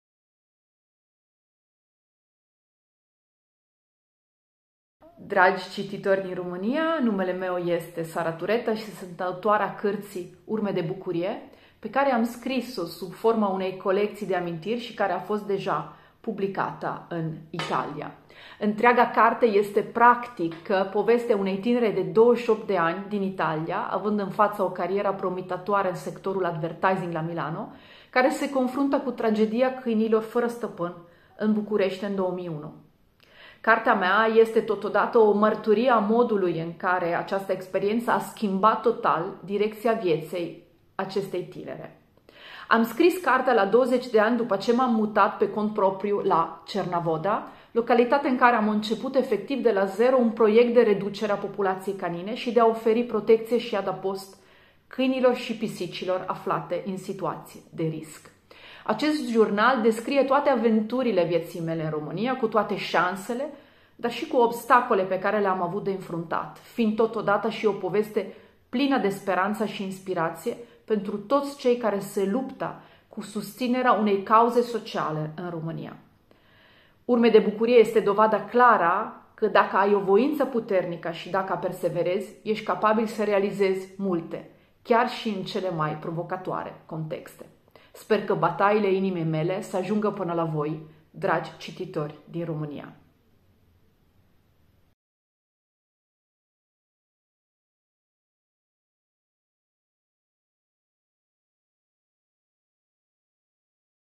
Sara Turetta prezintă cartea „Urme de bucurie și alte amintiri românești ale unei italience”
Sara Turetta, fondatoare și președintă a ONG-ului „Save the dogs and other animals”, prezintă cartea „Urme de bucurie și alte amintiri românești ale unei italience”, care spune povestea plină de obstacole, dar și de speranță a celor 20 de ani petrecuți în România. Volumul este publicat în colecția Memorii/Jurnale, în traducerea Lilianei Nechita.

„Cartea Sarei este despre curajul de a schimba lumea, despre lupta ei cu un sistem corupt și cu indiferența oamenilor. Într-un fel, m-a durut să văd că a deschis cutia Pandorei, de parcă acea parte urâtă ar trebui acoperită la nesfârșit. Dar știu că ne vom face bine numai dacă ne vom da seama că suntem bolnavi. Această carte e un bisturiu, taie în carnea vie a ideii că suntem grozavi și ne arată așa cum suntem, prea des nepăsători față de copii, bătrâni și animale. Este o lecție despre speranță și despre cum să nu cedezi atunci când crezi că totul e împotriva ta. Despre dorința de a lăsa urme. De fericire, desigur.“ — LILIANA NECHITA

„Dacă mă gândesc la acești douăzeci de ani petrecuți în România, îmi vin în minte cele 41.000 de animale sterilizate și suferința pe care am evitat o, iar imaginea celor peste 8.000 de câini adoptați în casele din Europa mă liniștește. Dar Save the Dogs a schimbat și viața celor care lucrează pentru asociație; a cetățenilor europeni care au primit un câine de la noi, vindecându se uneori de vreo boală sau de depresie; a sutelor de copii care vizitează centrul Urme de bucurie – în fiecare an le arătăm că animalele sunt tovarăși de drum, cu o mulțime de calități. A schimbat și viața multor români, care consideră oaza noastră un adevărat miracol. Lor aș vrea să le spun că luptele pentru o Românie mai dreaptă și mai echilibrată sunt și ale mele; chiar dacă văd lucrurile prin prisma drepturilor animalelor și a muncii la Save the Dogs, inima mea bate la unison cu inimile românilor care luptă să construiască o țară mai bună.
Sper ca bătăile inimii mele să ajungă până la voi, dragi cititori.“ — SARA TURETTA